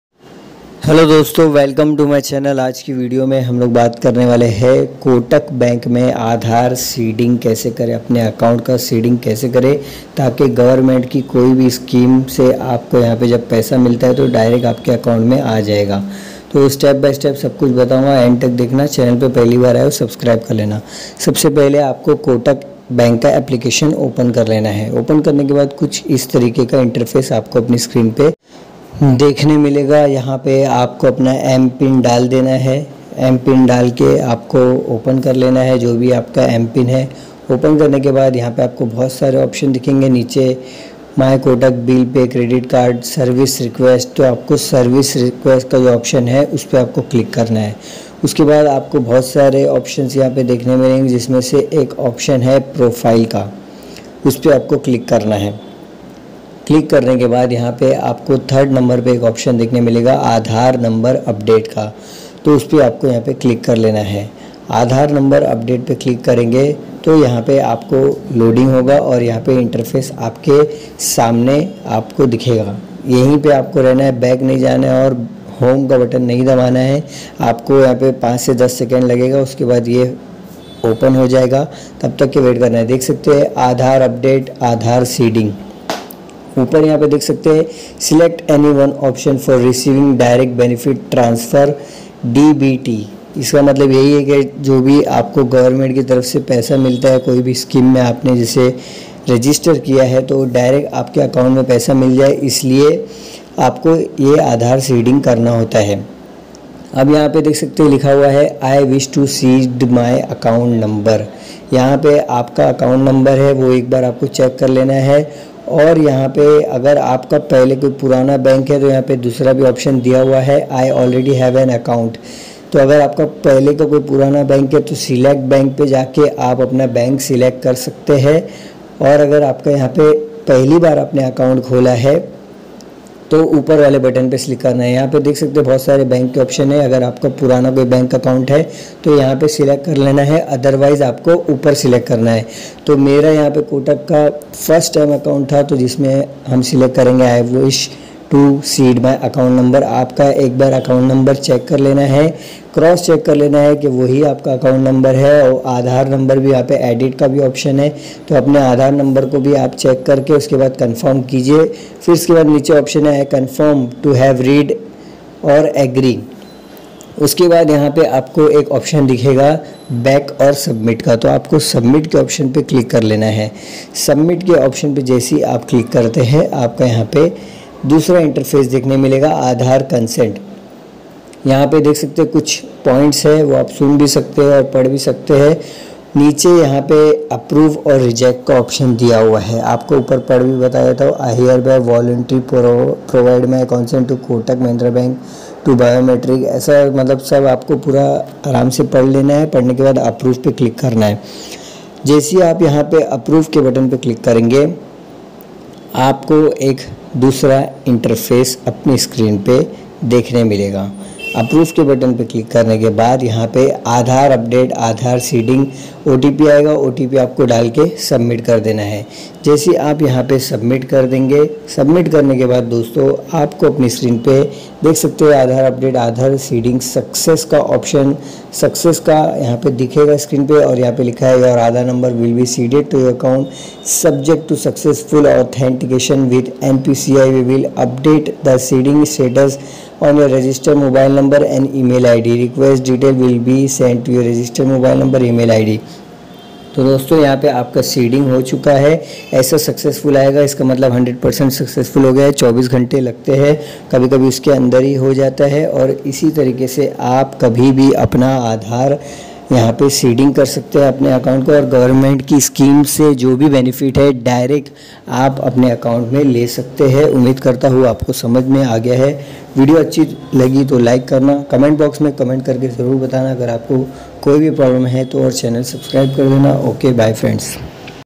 kotak mahindra bank me aadhar seeding kaise kare | how to seed aadhaar with kotak bank account
00:00 kotak mahindra bank me aadhar seeding kaise kare
00:30 kotak mahindra bank aadhar card link
01:30 how to seed aadhaar with kotak bank account
02:00 how to seed aadhaar with kotak bank account online

in this video I'm talking about kotak mahindra bank me aadhar seeding kaise kare